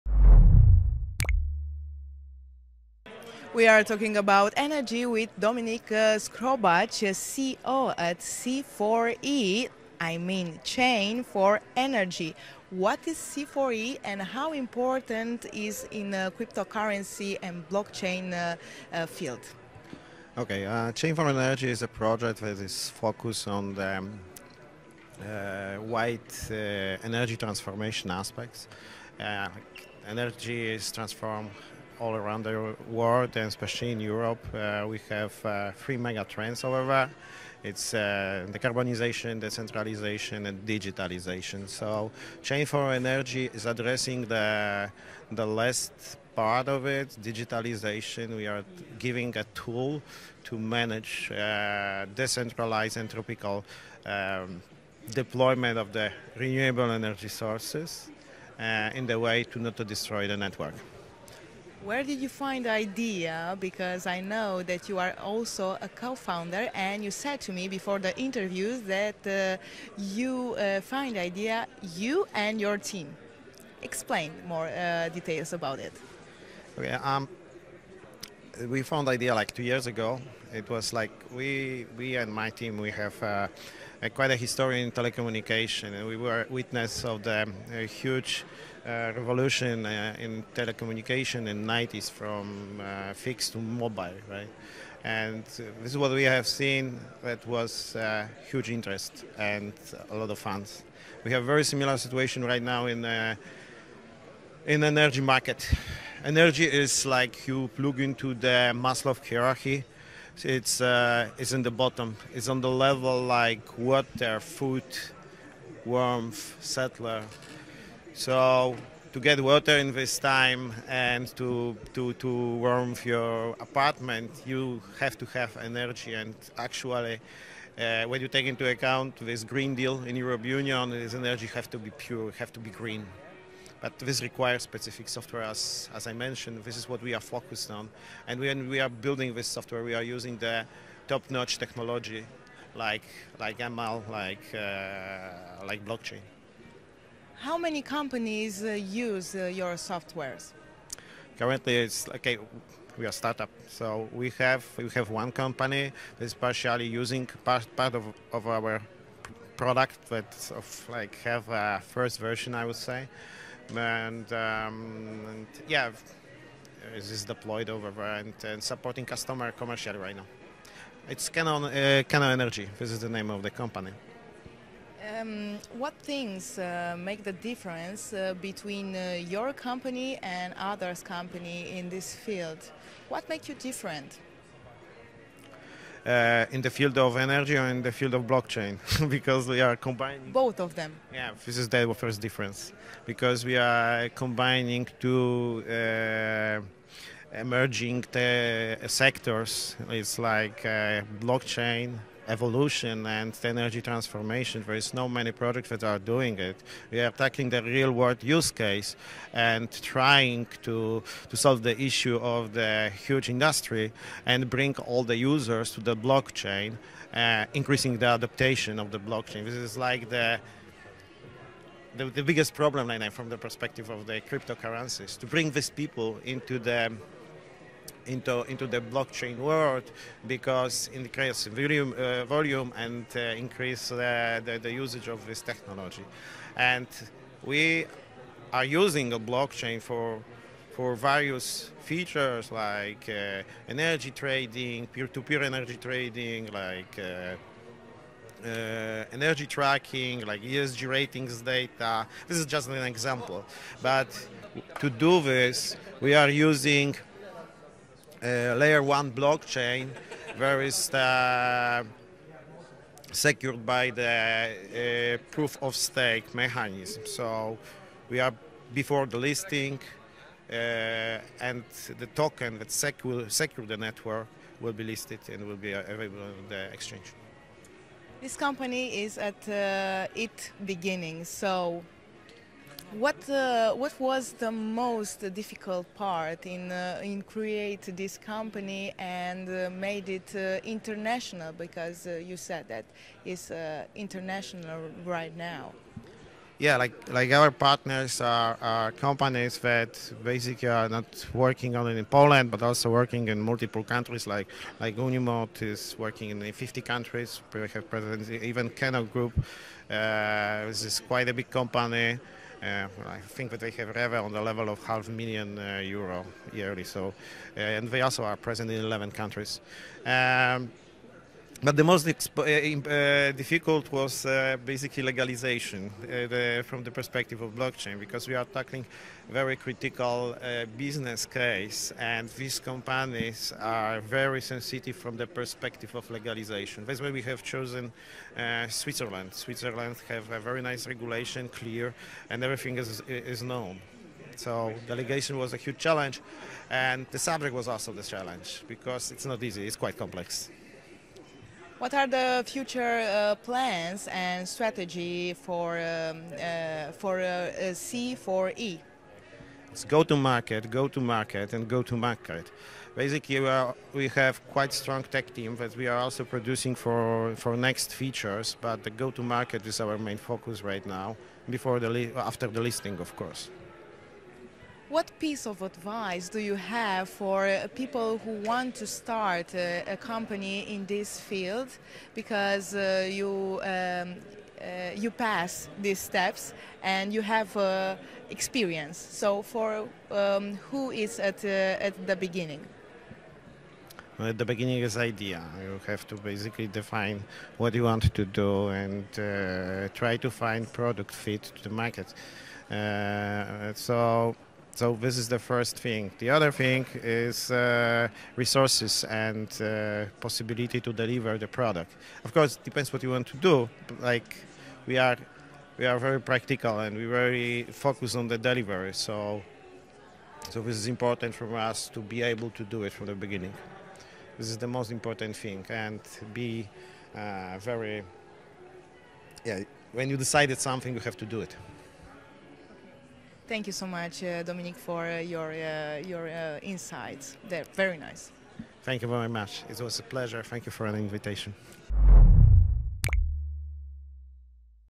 Dominik Skrobacz, Co-founder & CEO at C4E - NBX Warsaw Summit - CryptoKTV
CryptoKTV team conducting an interview with Dominik Skrobacz, Co-founder & CEO at C4E at the NBX Warsaw Summit
24-25 MAY 2023